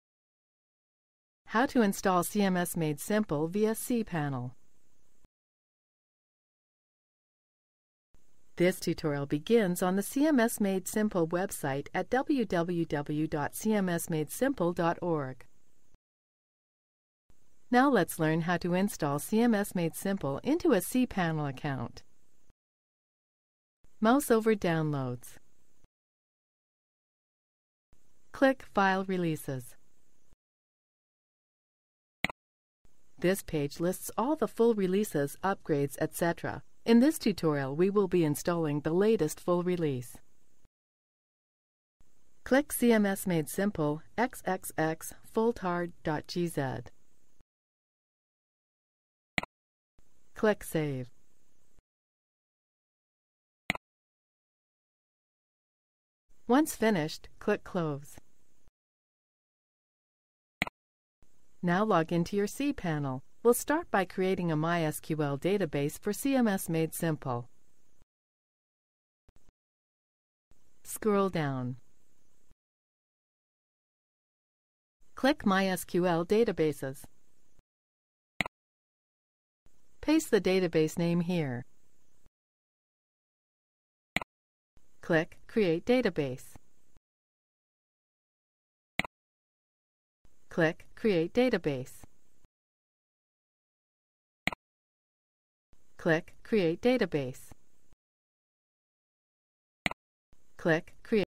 How to install CMS Made Simple via cPanel - Cms Tutorials
How to install CMS Made Simple via cPanel
• Select the downloads tab. The next window will show the all releases, upgrades etc.
• Click on any release and save it.
• Now log in to your cPanel. MYSQL database has to be created for CMS made simple.
• Click on MYSQL databases. Give a database name and click n Create Database button. Database will be created. Click on Go Back tab.
• Put user name, password and create a new user. To complete the database steps, it needs to be given a new name. Give a new name to DB and click on Add button.
• Choose All Privileges option and click on Make Changes button. Go Back and click on Home icon.
• MySql Database, user created and user assigned to new database.
• The next step will be to upload CMS made simple achieve to server and get ready for its installation.
• Check files menu and click on File manager option. Double click on public_html. Click the upload icon on top. The selection window will appear, from where you can browse the files to be uploaded. Once finished, get back to the previous directory by tab given below.
• Scroll down and click on the file archive, verify the extraction path. Click on �Extract files� when finished the task.
• The next step is to make certain directories writable. Select the files and click on change permissions options. Give them the permissions desired by you. You can notice the new permissions, mentioned with file names. You can also do it for templates, images and cache settings.
on your browser. Click on Continue button for a new installation. Select the language of your operation. Click on continue button.
• Click on New file icon. Name the file config.php and change its permissions (as already explained above).
• Scroll down and check for File Creation Mask.
• Create admin account information. Scroll down. The next step is to enter DB information in the text boxes provided. Click on continue button. Tables of DataBase will be created successfully.
• The installation will be done and you can start using CMS from cPanel.